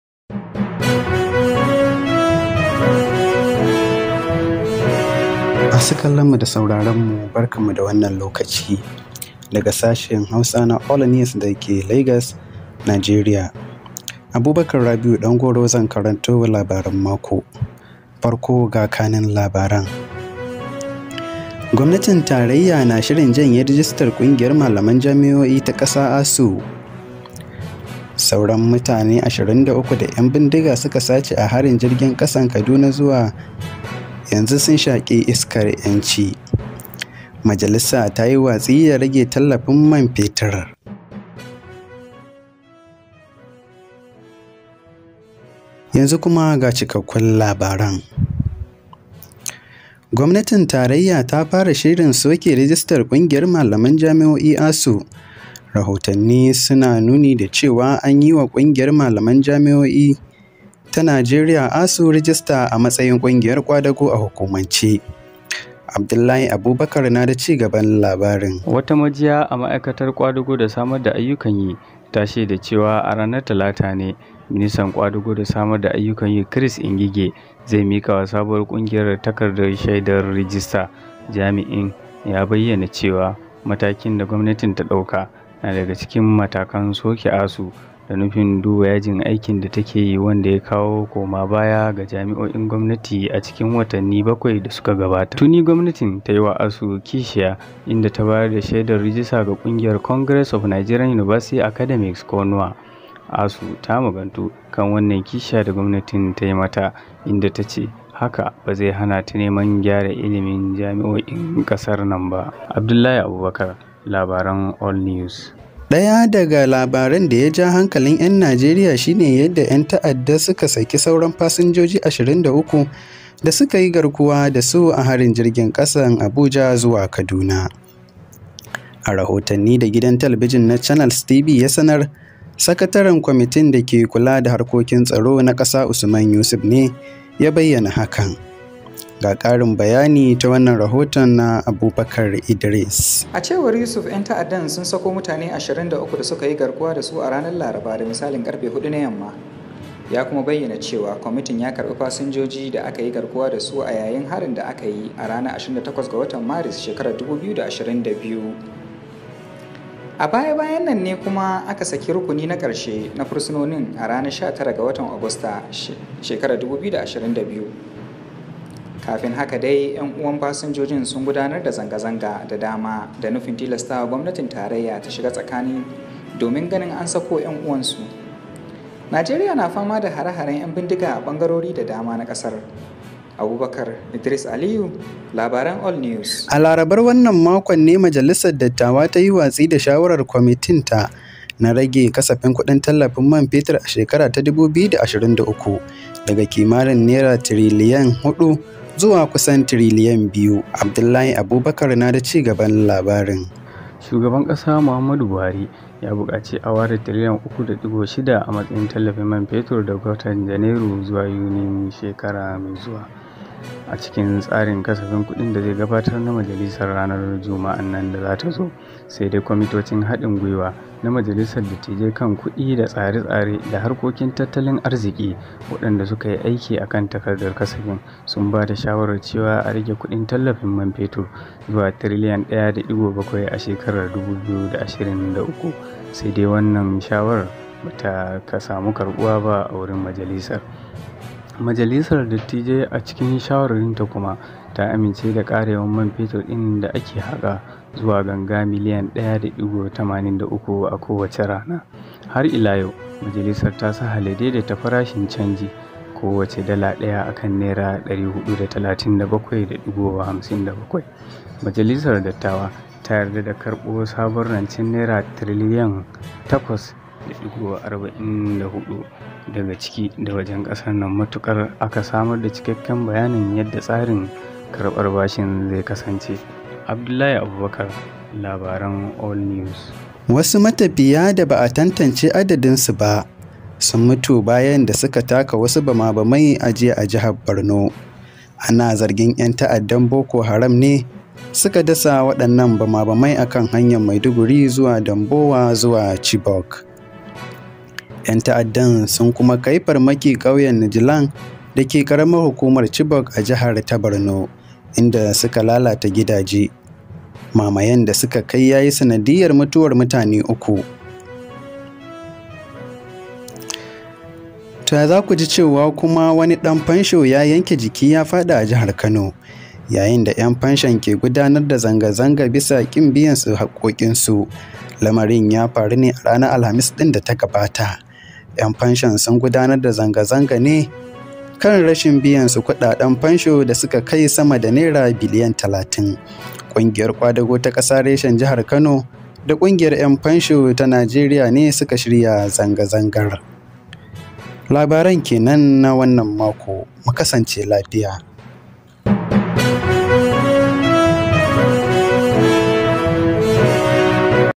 FG plans to withdraw ASUU's registration |Nigerian senate refuses proposed reduction of fuel subsidy
Hausa News Today 7 October, 2022

Gwamnatin Tarayya Na Shirin janye rajistar kungiyar malaman jami'o'i ta kasa, ASUU.

Sauran mutane 23 da ƴam Bindiga suka sace a harin jirgin kasan Kaduna sun shaki iskar ƴanci.

Majalisa ta yi watsi da rage tallafin man fetur.

Wasu matafiya da ba'a kai ga tantance su nawa ne ba sun mutu bayan taka bama-bamai a Borno.

Wani ɗan fansho ya yanke jiki ya fadi yayin zanga zangar neman hakkin su a Kano.

FG plans to withdraw ASUU's registration, registers new University union.

Remaining 23 Kaduna Train attack victims finally released by their captors.

Nigerian senate refuses proposed reduction of fuel subsidy.

Unknown number of travellers die in Borno after stepping on bombs.

Pensioner Slumps during protest over unpaid pension in Kano.